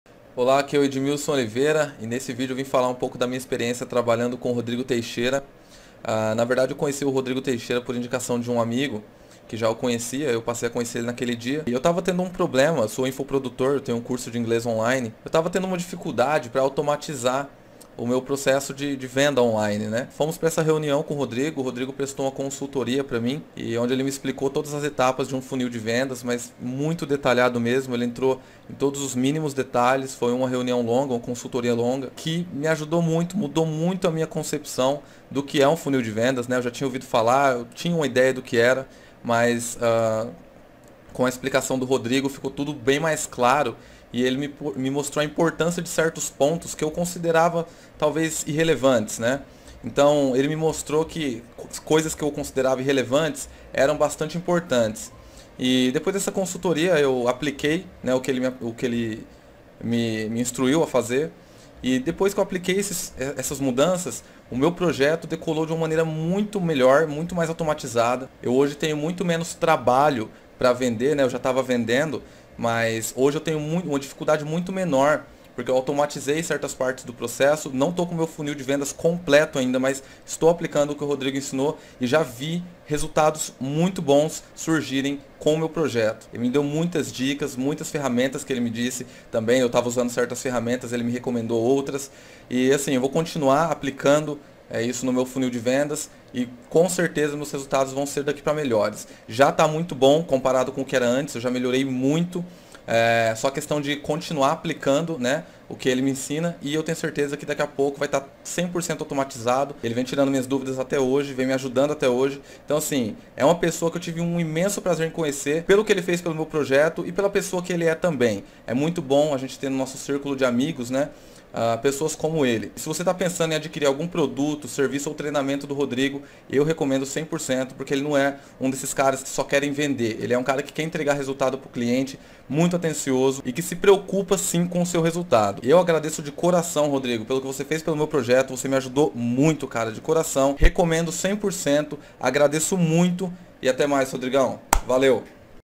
Depoimento do Edmilson do "Ingles do Ed' Para o Rodrigo
Mais um depoimento de aluno/cliente satisfeito. Obrigado Ed pelas palavras!